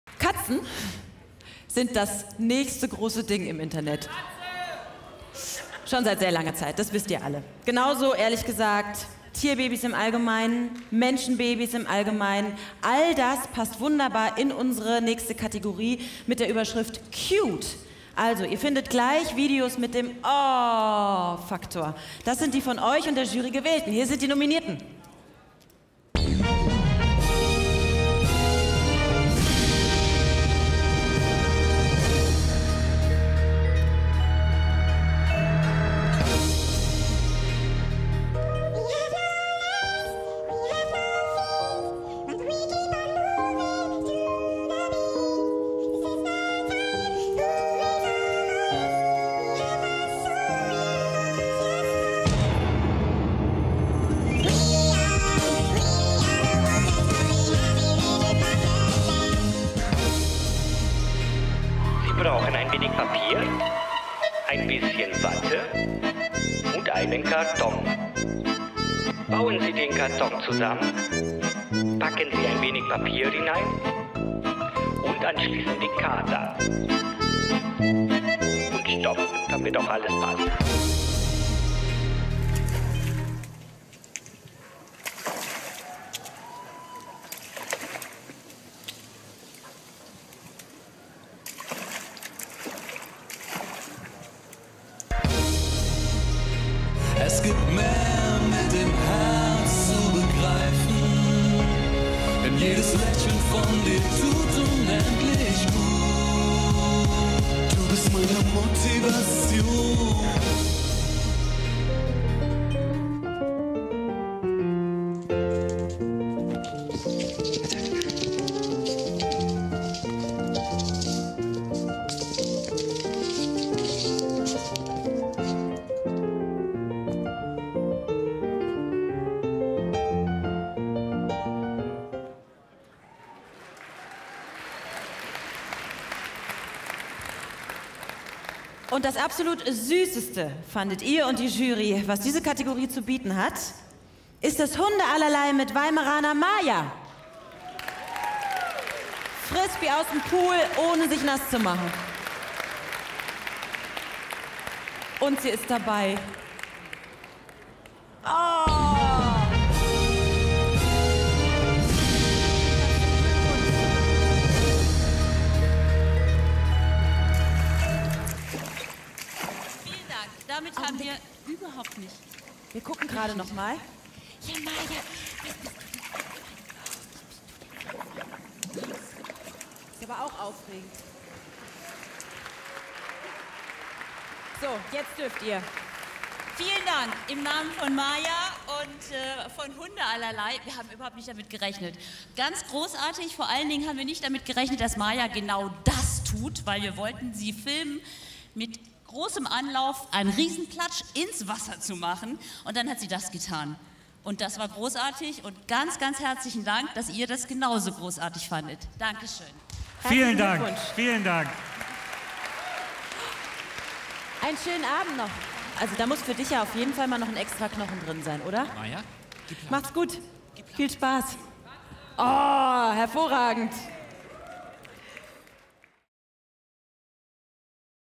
Maya Weimaraner - Gewinner Cute - Deutscher Webvideopreis 2013
Von Catcontent bis zum Babylachen: bei Cute geht es um das Webvideo mit dem größten „Ach, ist das süß"-Wohlfühlfaktor des Jahres.

Gewinner:

Alice Häuser - Maya Weimaraner kann die Frisbee auch ohne ins Wasser zuspringen, aus dem Pool holen